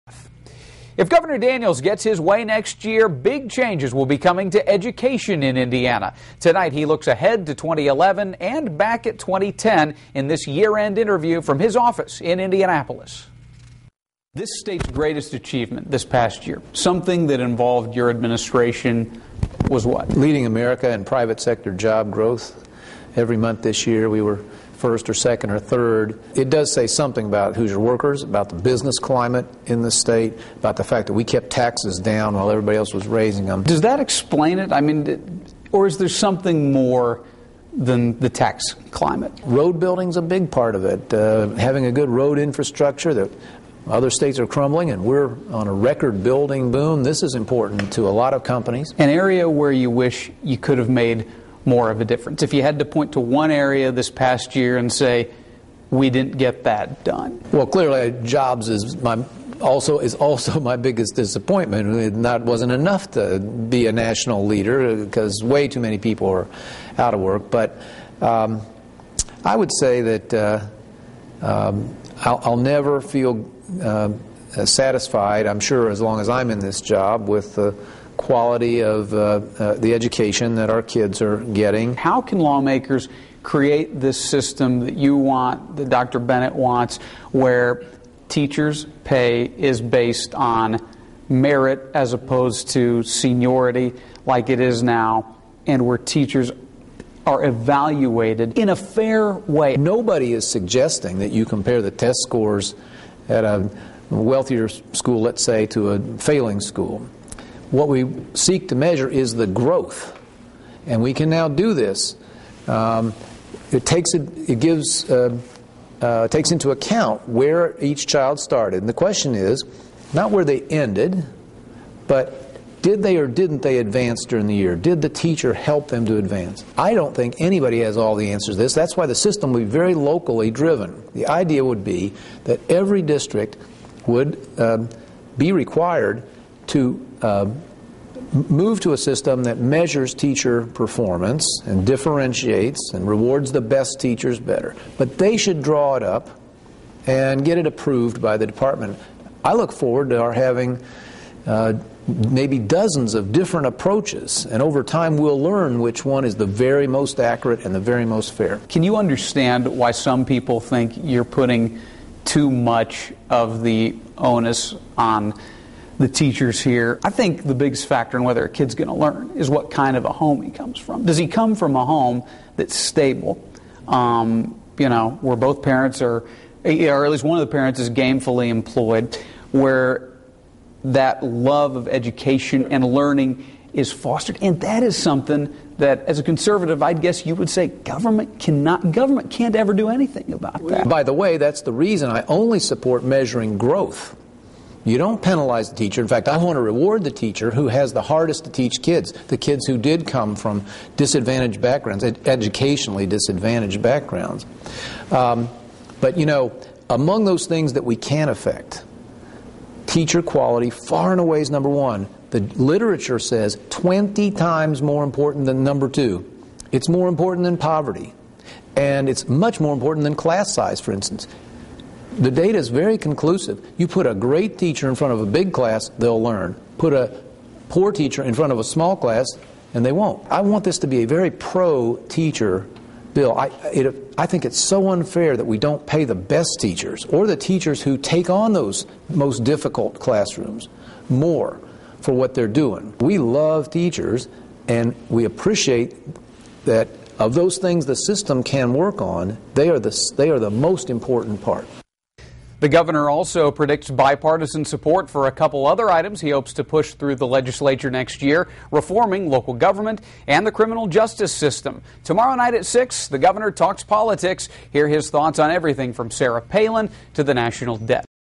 Daniels Talks about big changes to Indiana Education
NewsChannel 15 does a year-end interview with Governor Mitch Daniels where he talks about the big changes that will be coming to education in Indiana.